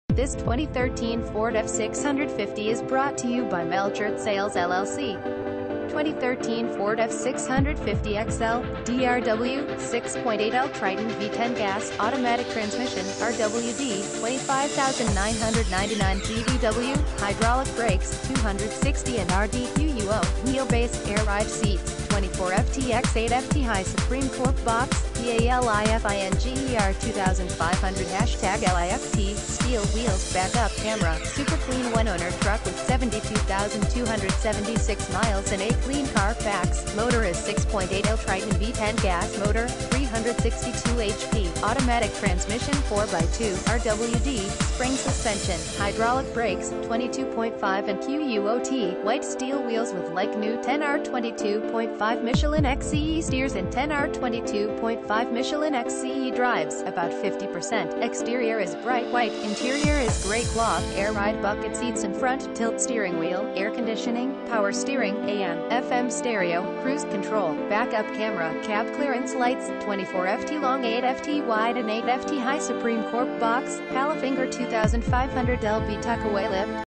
2013 FORD F650 XL V10 GAS AUTO 24' BOX LIFT GATE BCAM ONLY 72K MILES (Neenah, Wisconsin)
Melchert Sales LLC
1919 American Ct
Neenah